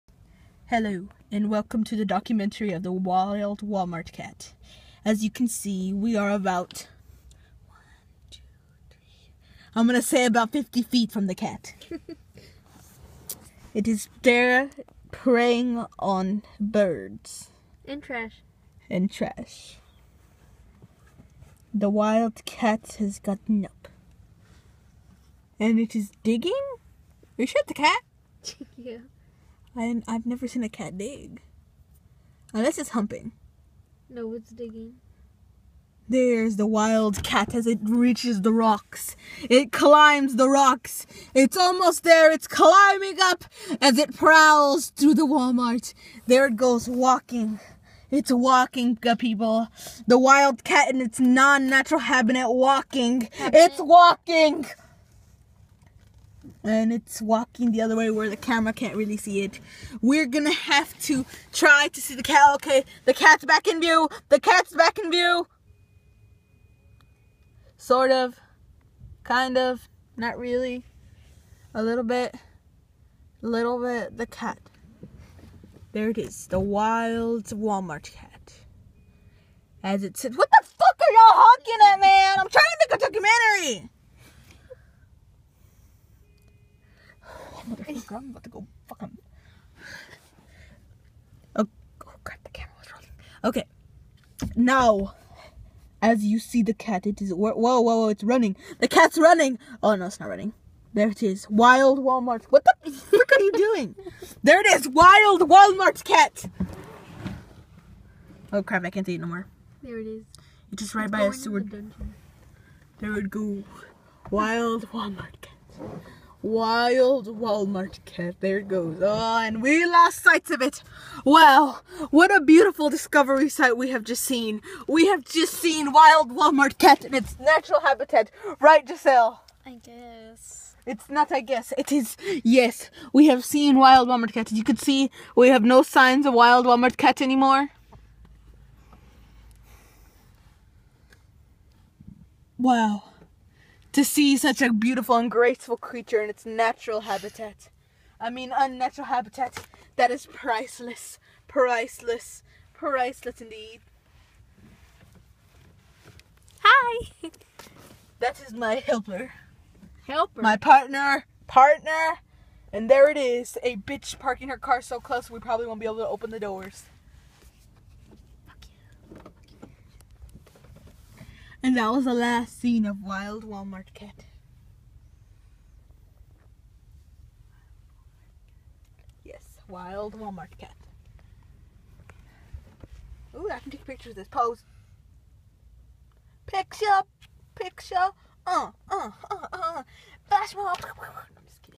Wild Walmart Cat "The documentary"
One of the most glorious creatures in its unnatural habitat... THE WALMART CAT!!!